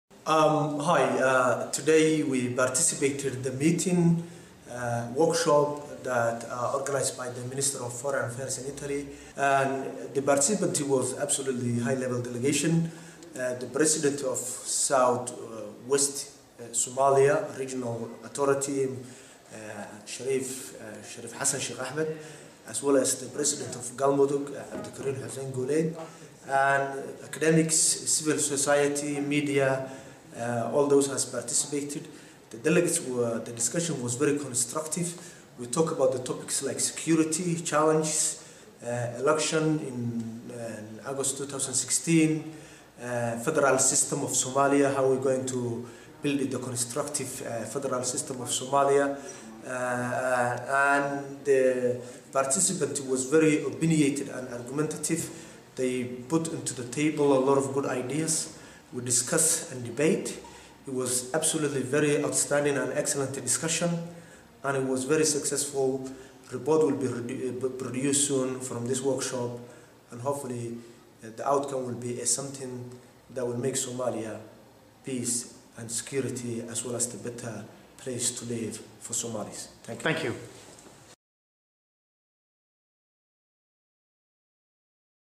Ali Dahir - Somali perspectives: institutional and policy challenges
Interview with Ali Dahir - Senior executive Shabelle Media Network - during the meeting "Somali perspectives: institutional and policy challenges" organized by IAI.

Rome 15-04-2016